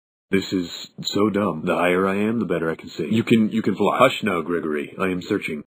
hush now gregory
I haven't animated ponies in a long time and it shows haha

yikes

Also you can tell how burnt out I got in the end